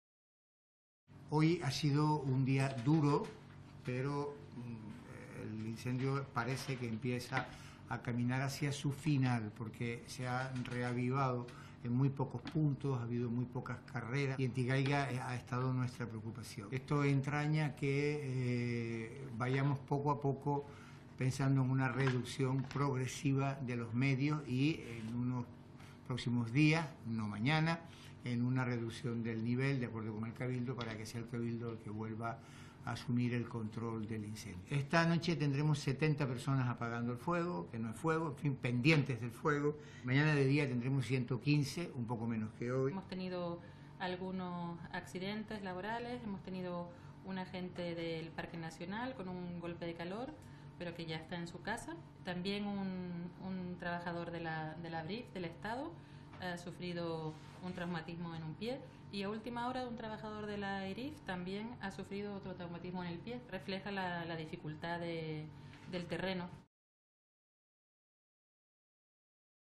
Pérez: "Hoy ha sido un día duro, pero el incendio parece encaminarse hacia su final"
Declaraciones de Julio Pérez, consejero de Administraciones Públicas, Justicia y Seguridad del Gobierno de Canarias; y de Marta Moreno, subdirectora de Protección Civil, en la rueda de prensa de este martes, 26 de julio, sobre el incendio de Tenerife.